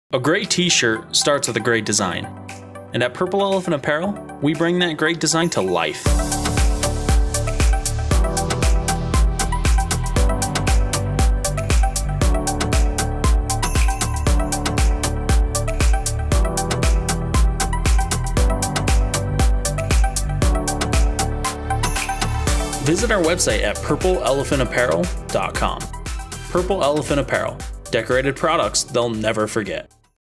Design Your Own T-shirts At Purple Elephant Apparel
Bring your own designs to life at Purple Elephant Apparel. to start designing today.

We believe that just because you use our online t-shirt designer, instead of ordering face to face, doesn't mean that you shouldn't get all the help and attention you need. Our product and customizing experts will go the extra mile to make sure you get what you want and answer all of your questions, even if it is for just one custom t-shirt or several customized sweatshirt.